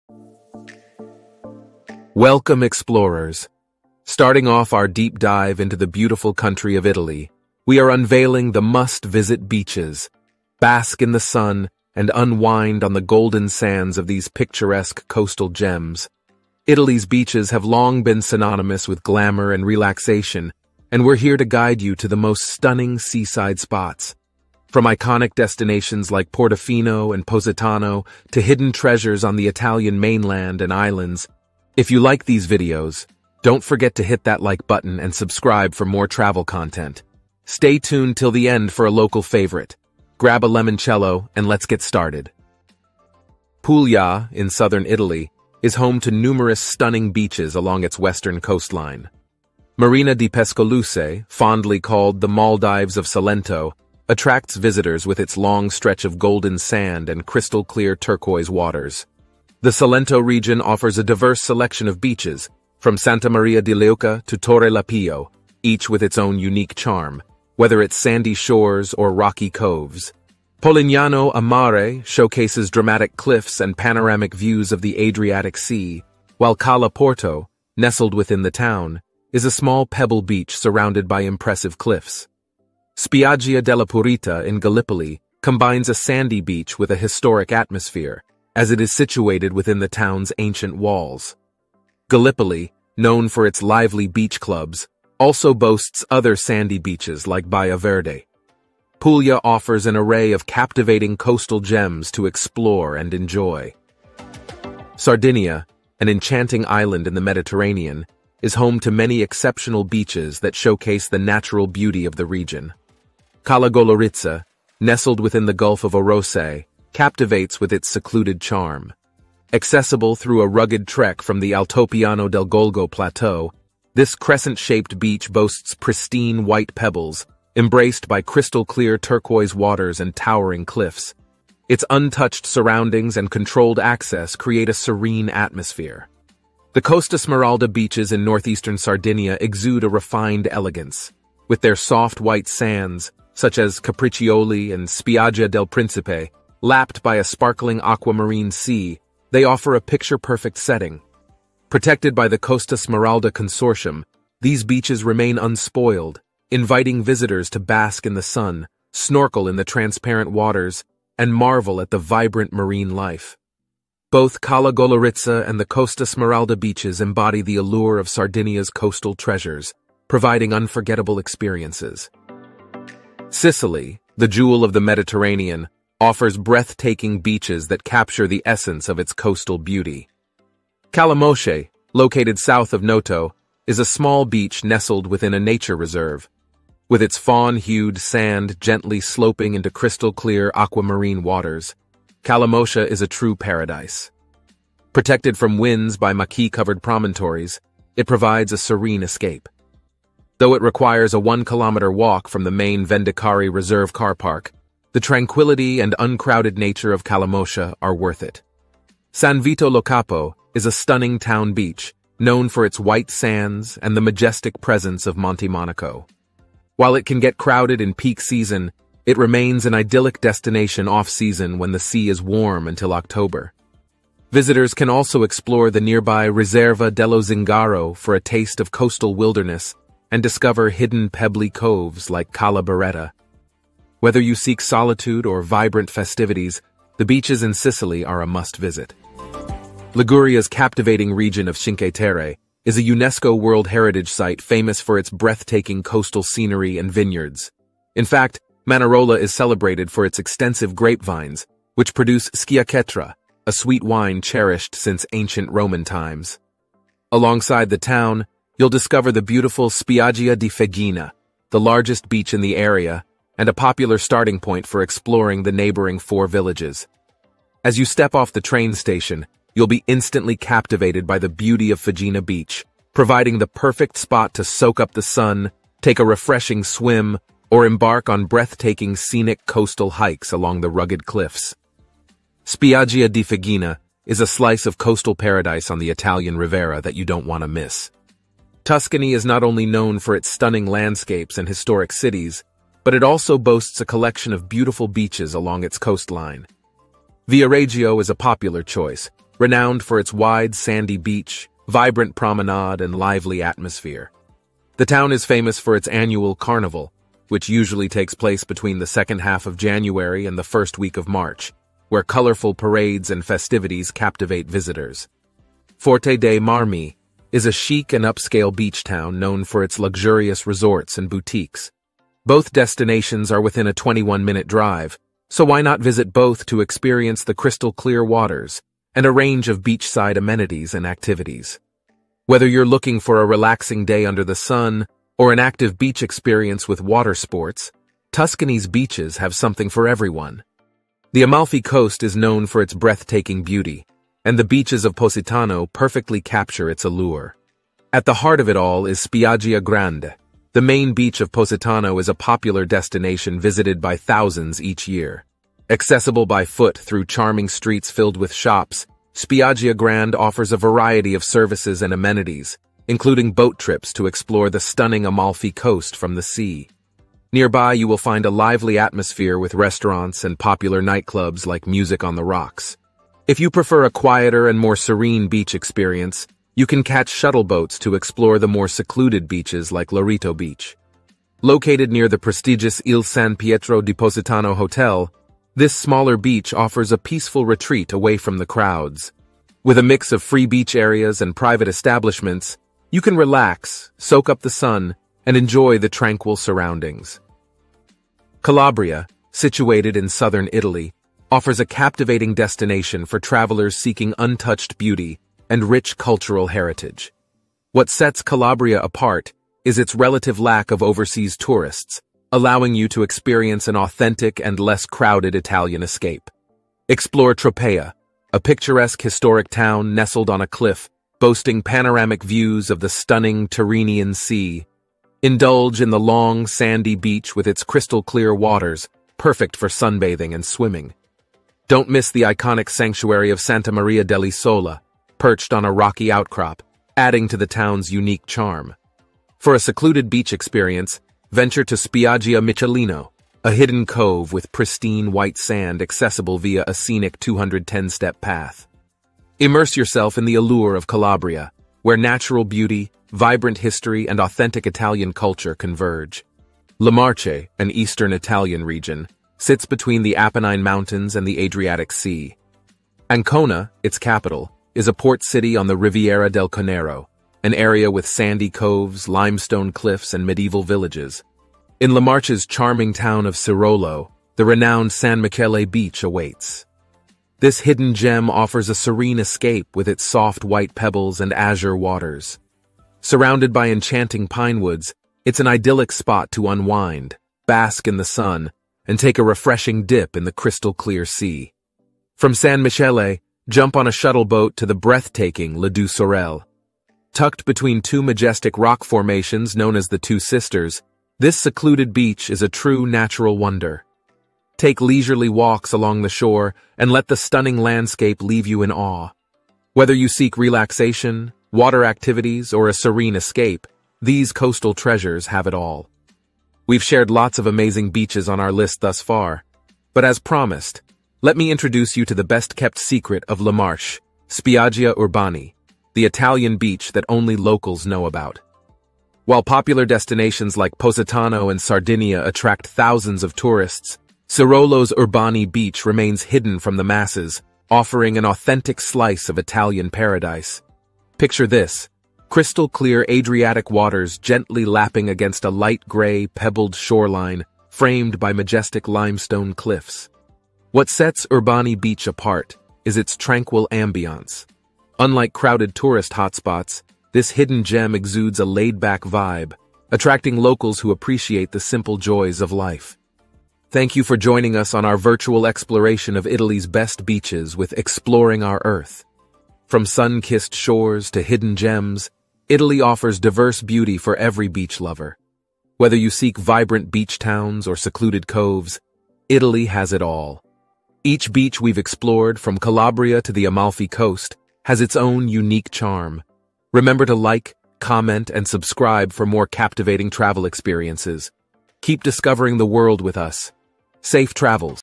Best Beaches in Italy | Your Ultimate Travel Guide 2023🇮🇹
Looking for the best beaches? Look no further than Italy! With its stunning beaches and endless scenic vistas, Italy is the perfect destination!

In this video, we're going to share with you the ultimate guide to Italy's stunning beaches. From popular destinations to tips on how to get the most out of your trip, this video will help you plan the perfect trip to Italy's beautiful beaches!

We want you to be part of our adventure! The world is waiting to be explored, and we're thrilled to have you on this journey with us. Let's dive into the wonders of Italy's stunning coastlines together!
0:00 Intro
00:43 Puglia
01:45 Sardinia
02:53 Sicily
04:00 Liguria
04:48 Tuscany
05:14 Amalfi Coast
06:39 Calabria
07:38 Le Marche
08:36 Secret beach and Outro